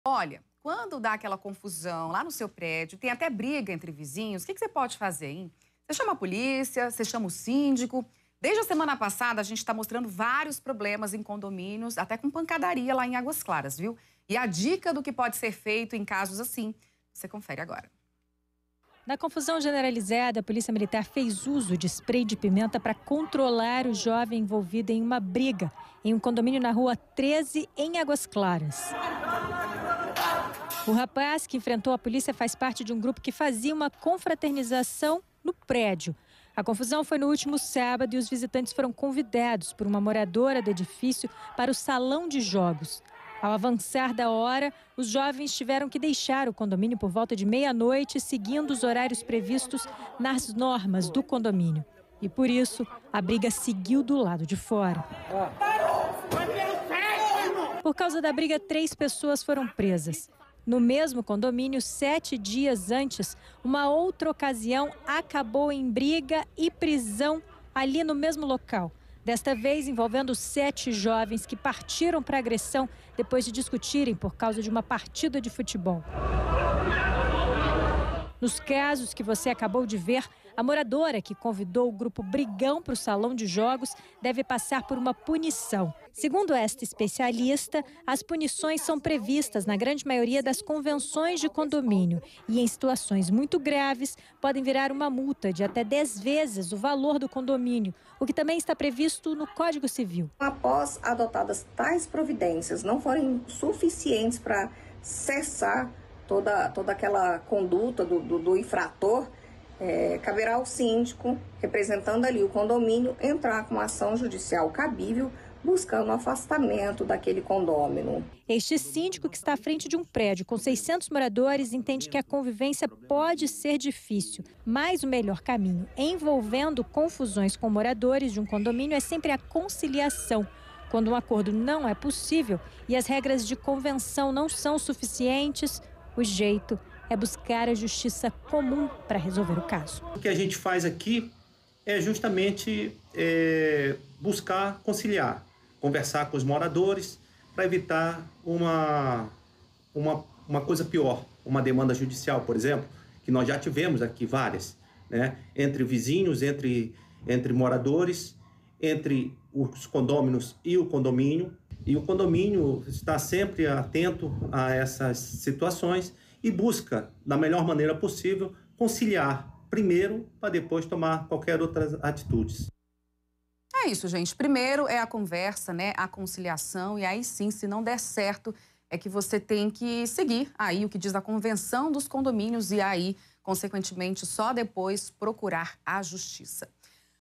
Aprenda a melhor forma de resolver problemas em condomínio
Após o registro de brigas em condomínios de Águas Claras (DF) por causa de barulho, aprenda com o especialista a melhor forma de resolver conflitos em condomínios.